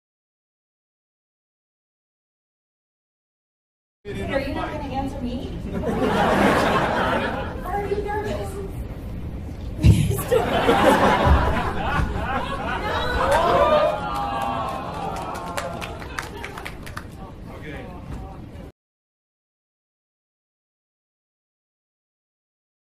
Shannen Hugs are awesome!
Shannen helps a fan not be nervous!

Video courtesy of @shannencomesaholic